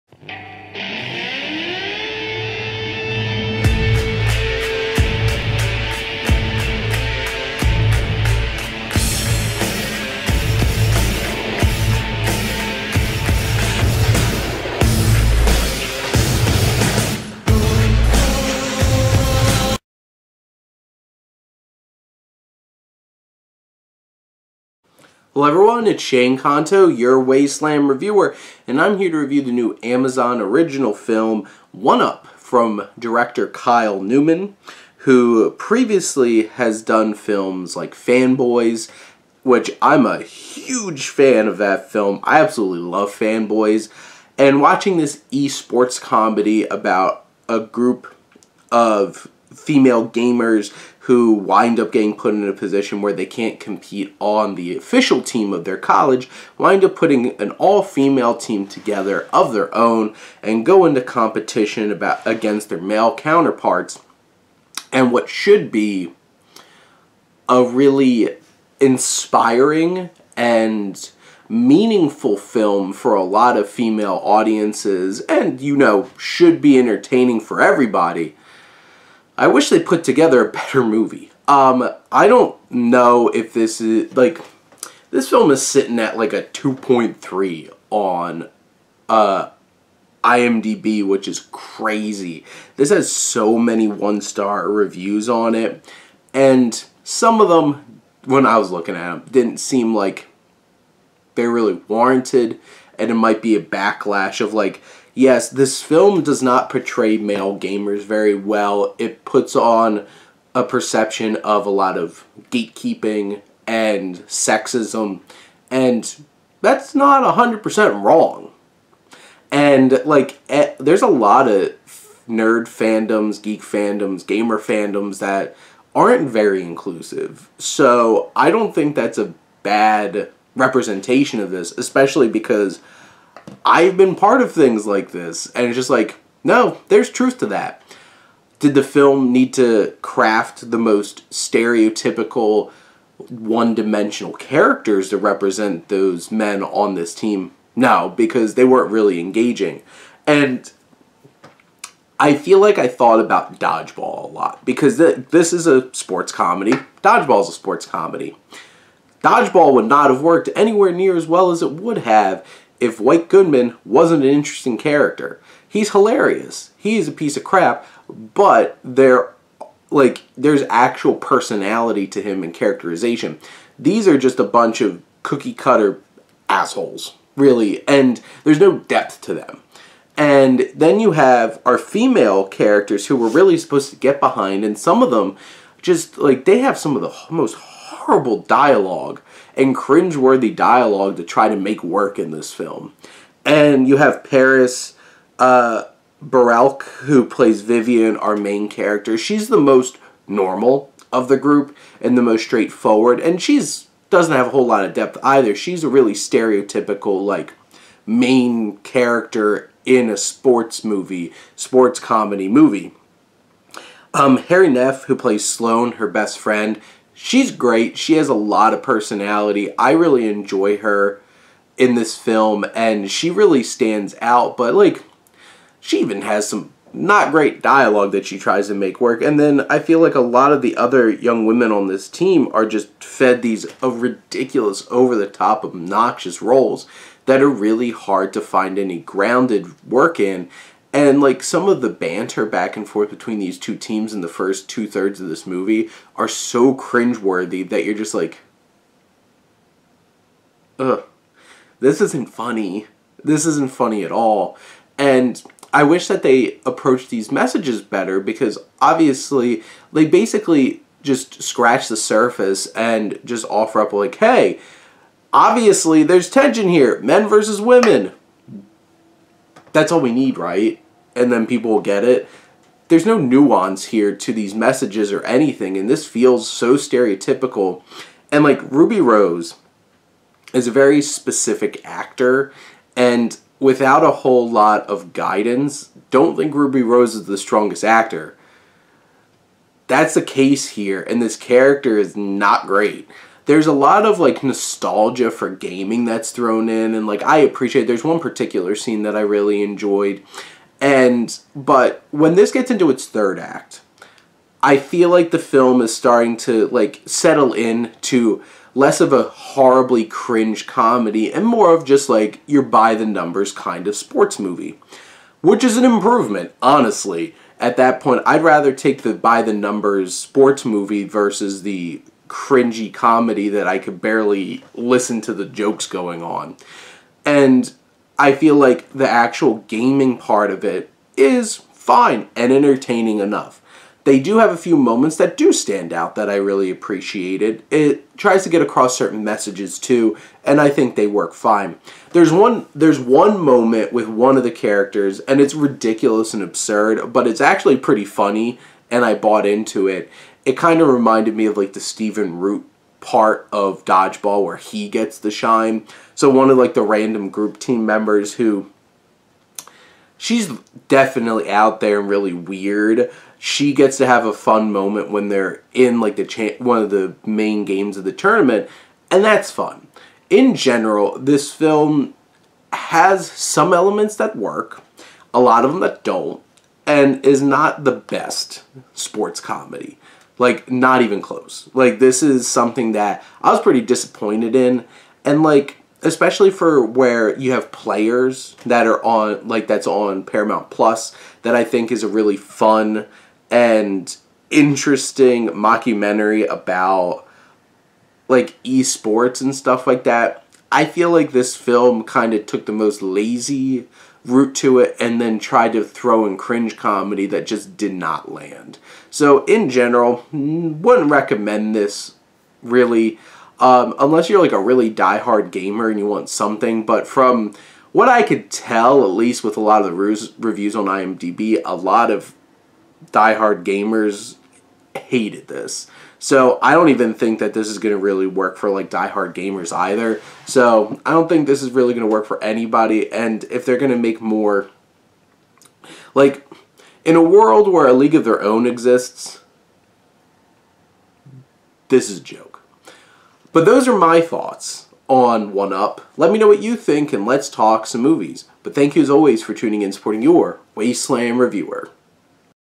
1UP (2022) - Wasteland Film Review
My Wasteland Review of 1UP
Directed by Kyle Newman
Starring: Ruby Rose, Paris Berelc, Hari Nef
Prime Video
Synopsis: Follows a gamer who quits her college esports team due to sexism from her male counterparts.